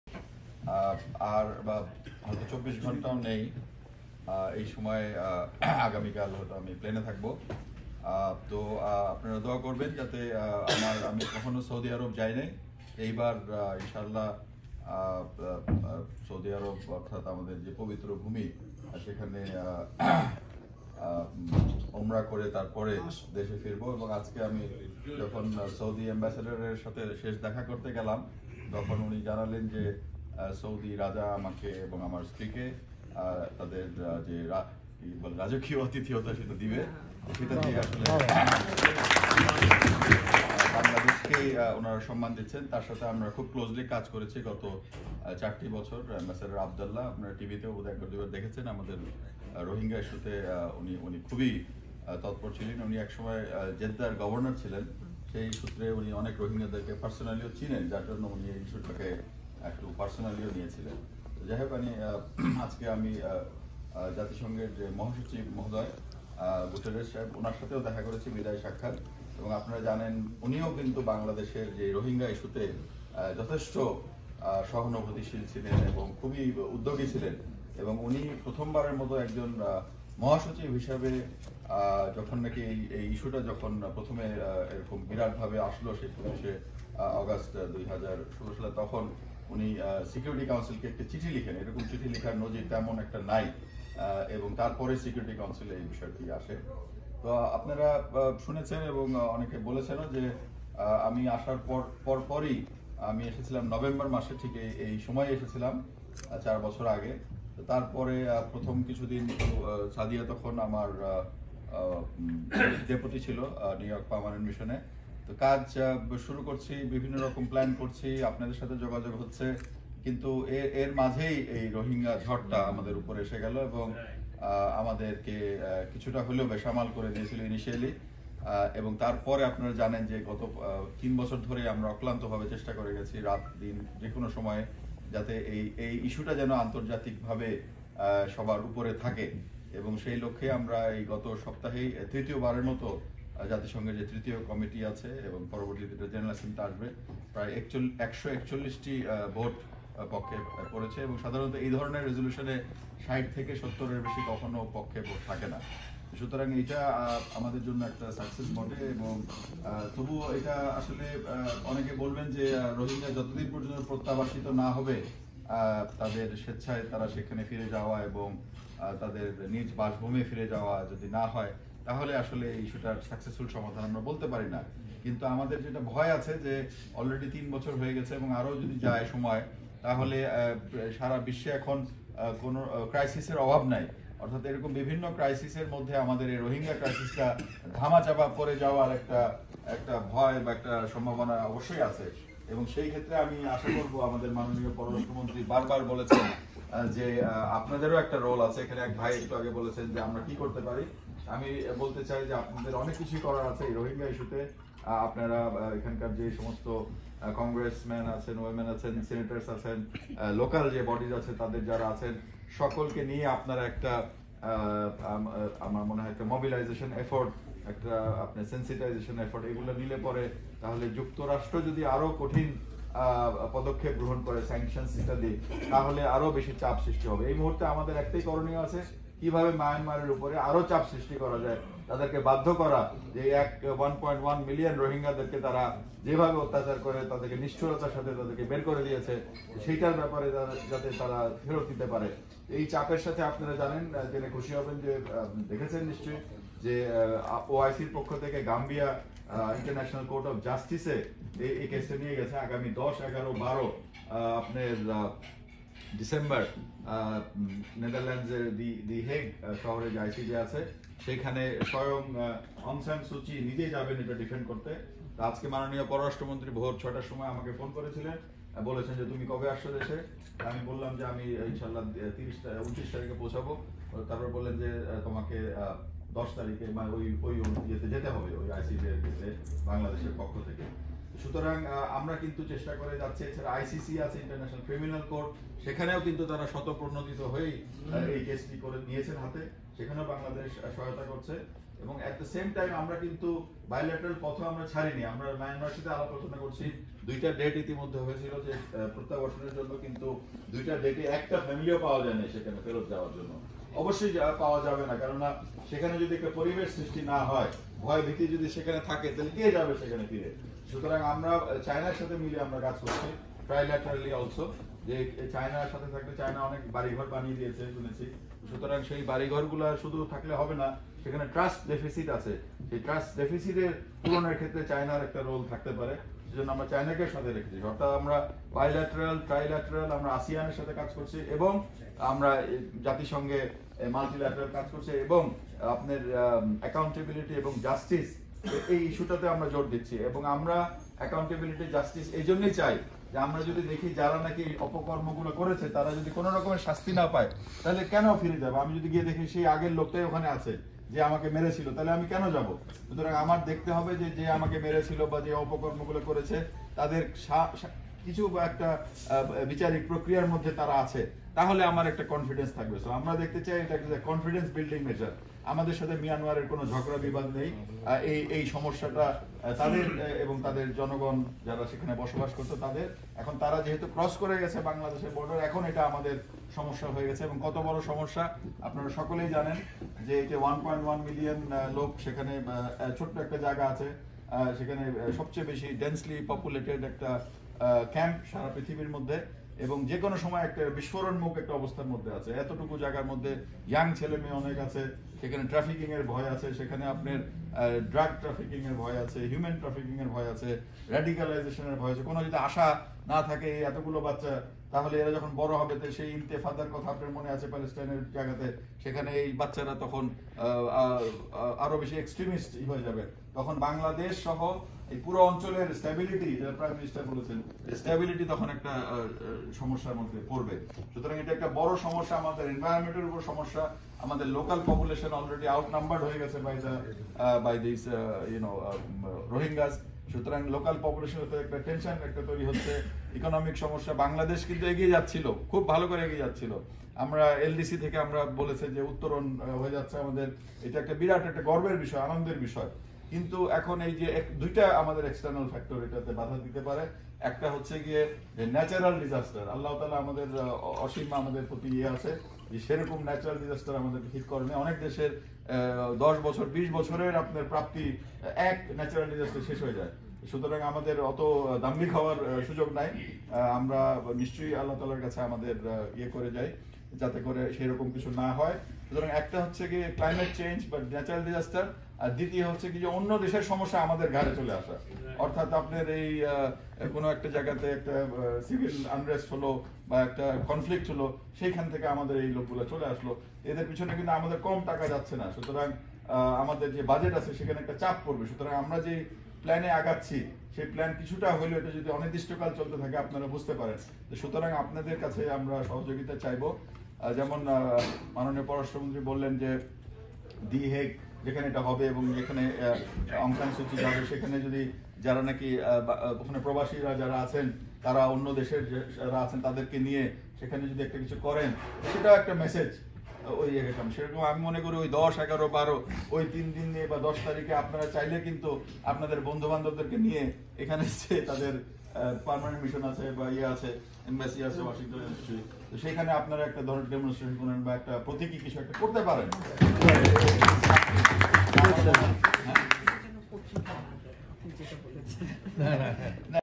A True Evening With Ambassador Masud Bin Momen : New York, USA, November 21, 2019
A True Evening With
Ambassador Masud Bin Momen
Speaking : Masud Bin Momen, Permanent Representative (UN ambassador) the Permanent Mission of Bangladesh to the United Nations in New York.
Ali Baba Party Hall, New York
Organized By: Bangladeshi-American Community, New York